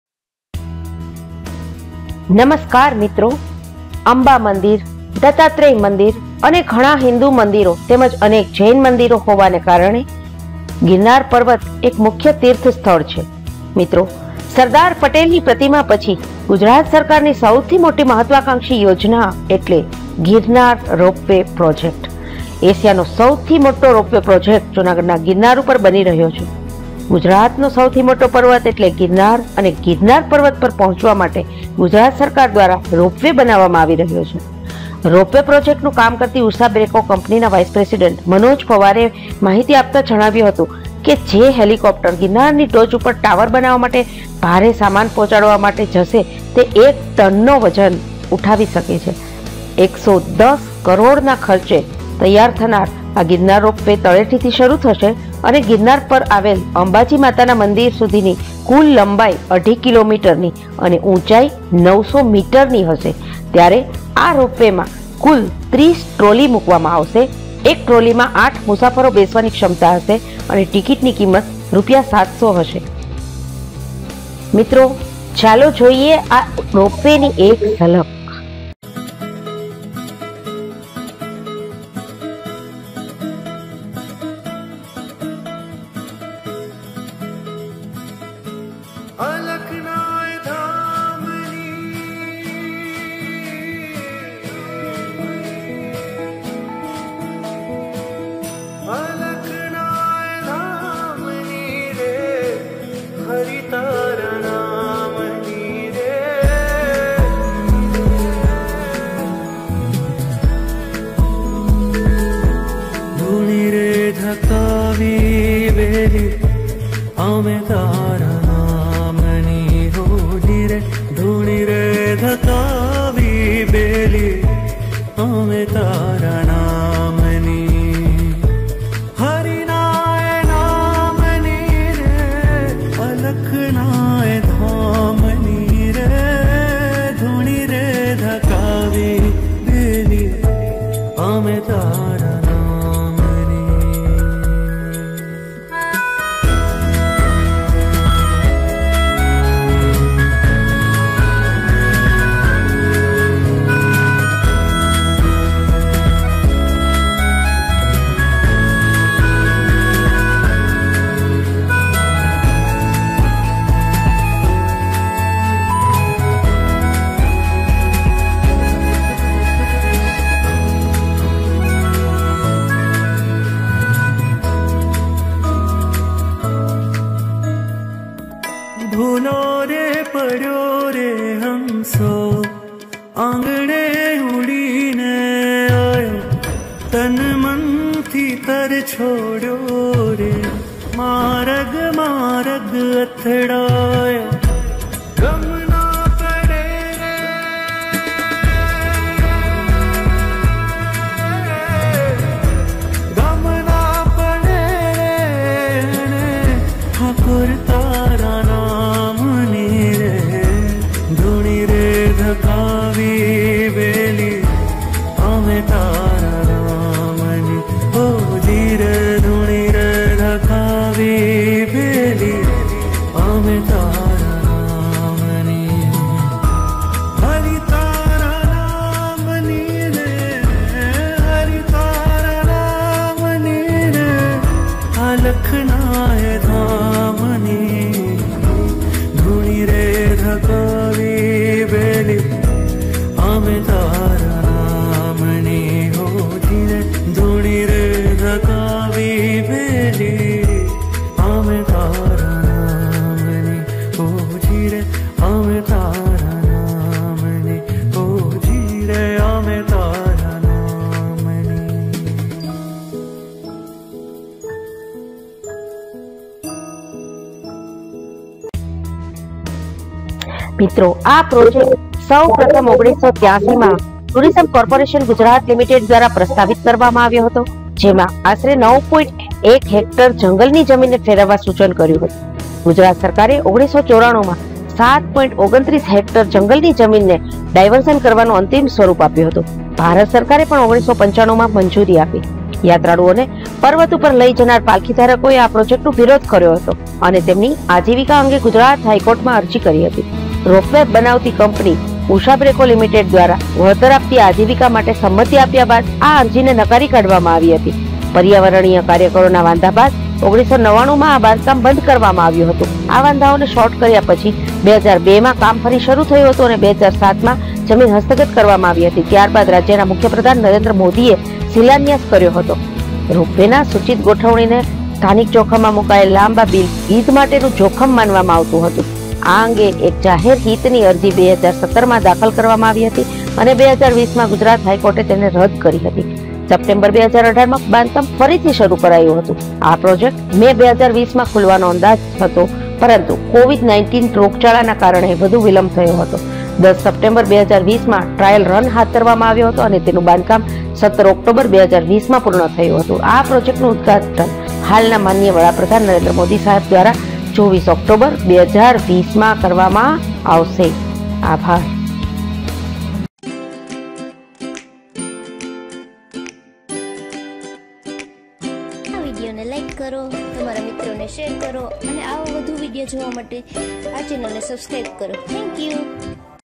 Girnar_Ropeway Junagadh_Girnar_Taleti Biggest_Asian_Ropeway
Presented By:Jayshree M. Vekaria
Prepared by: Dr. Mansukh V. Vekaria
રોપ વે - દ્વિમાર્ગી સેવા પ્રદાન કરશે, જેનો અર્થ છે કે મુસાફરો પણ તેનો ઉપયોગ ટેકરીની ટોચ પરથી પાછા ફરવા માટે કરી શકે છે. સેવાનો ઉપયોગ કરતા મુસાફરોને ગિરનાર પહોંચ્યા પછી ગ્રાઉન્ડ સ્ટેશન પર પાછા ફરવા માટે એક કલાક આપવામાં આવશે. ટિકિટની કિંમત રૂ .700 હોવાની સંભાવના છે
અંબા મંદિર, દત્તાત્રેય મંદિર અને ઘણા હિન્દુ મંદિરો તેમજ અનેક જૈન મંદિરો હોવાને કારણે ગિરનાર પર્વત એક મુખ્ય તીર્થસ્થળ છે.
આ પ્રોજેક્ટ સૌ પ્રથમ 1983 માં ટૂરિઝમ કોર્પોરેશન ગુજરાત લિમિટેડ (ટીસીજીએલ) દ્વારા પ્રસ્તાવિત કરવામાં આવ્યો હતો, જેમાં આશરે 9.1 હેક્ટર (22 એકર) જંગલની જમીનને ફેરવવા સૂચન કર્યું હતું. ગુજરાત સરકારે 1994 માં 7.29 હેક્ટર (18.0 એકર) જંગલની જમીનને ડાઇવર્ઝન કરવાનું અંતિમ સ્વરૂપ આપ્યું હતું અને તોરણિયા ગામ નજીક વળતર આપતી જંગલની જમીનને મંજૂરી આપવામાં આવી હતી. ભારત સરકારે પણ 1995 માં મંજૂરી આપી.
યાત્રાળુઓને પર્વત ઉપર લઈ જનાર પાલખી ધારકોએ આ પ્રોજેક્ટનો વિરોધ કર્યો હતો અને તેમની આજીવિકા અંગે ગુજરાત હાઇકોર્ટમાં અરજી કરી હતી. કંપનીએ રોપ-વે બનાવતી કંપની ઉષા બ્રેકો લિમિટેડ દ્વારા વળતર આપતી આજીવિકા માટે સંમતિ આપ્યા બાદ આ અરજીને નકારી કાઢવામાં આવી હતી. 2020 સુધીમાં, પ્રોજેક્ટ દ્વારા અસરગ્રસ્ત પાલક-વાહકો માટેની કુલ 104 દુકાનો, નીચલા ટર્મિનલના પાર્કિંગમાં બનાવવામાં આવી રહી છે. પર્યાવરણીય કાર્યકરોના વાંધા બાદ, 1999 માં આ બાંધકામ બંધ કરવામાં આવ્યું હતું.
આ વાંધાઓને સોર્ટ કર્યા પછી 2002 માં કામ ફરી શરૂ થયું હતું અને 2007 માં જમીન હસ્તગત કરવામાં આવી હતી. ત્યારબાદ રાજ્યના મુખ્ય પ્રધાન નરેન્દ્ર મોદીએ શિલાન્યાસ કર્યો હતો. પ્રોજેક્ટ ગિરનાર વન્યપ્રાણી અભયારણ્ય અને રાષ્ટ્રીય ઉદ્યાનની અંદર હોવાથી વન્યપ્રાણીઓની મંજૂરીને રાષ્ટ્રીય મંડળ દ્વારા વન્યપ્રાણીજીવન ૨૦૧૧ માં આપવામાં આવી હતી, જ્યારે પર્યાવરણીય મંજૂરી, ભારતના પર્યાવરણ, વન અને હવામાન પરિવર્તન મંત્રાલયે 2016 માં આપી હતી.
રોપ-વેના સૂચિત ગોઠવણીને સ્થાનિક જોખમમાં મૂકાયેલા લાંબા બીલ ગીધ માટેનું જોખમ માનવામાં આવતું હતું. આ અંગે એક જાહેર હિતની અરજી 2017 માં દાખલ કરવામાં આવી હતી અને 2020 માં ગુજરાત હાઇકોર્ટે તેને રદ કરી હતી.

સપ્ટેમ્બર 2018 માં બાંધકામ ફરીથી શરૂ કરાયું હતું. આ પ્રોજેક્ટ મે 2020 માં ખુલવાનો અંદાજ હતો પરંતુ COVID-19 રોગચાળોને કારણે વધુ વિલંબ થયો હતો. ૧૦ સપ્ટેમ્બર 2020 માં ટ્રાયલ રન હાથ ધરવામાં આવ્યો હતો અને તેનું બાંધકામ 17 ઓક્ટોબર 2020 માં પૂર્ણ થયું હતું. આ પ્રોજેક્ટનું ઉદઘાટન હાલના માન.વડા પ્રધાન નરેન્દ્ર મોદી સાહેબ દ્વારા 24 ઓક્ટોબર 2020 માં કરવામાં આવશે.